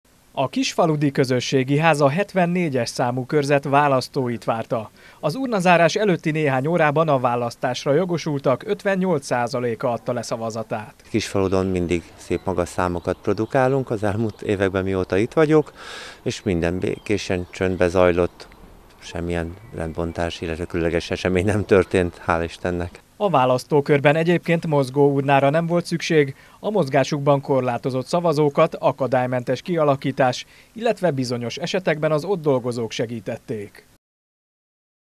A KISFALUDI KÖZÖSSÉGI HÁZ IS VÁRTA A VÁLASZTÓKAT
A Kisfaludi Közösségi Házban is a reggeli óráktól kezdve várták a szavazókat. Fél hétig a választók 65,55 %-a élt szavazati jogával.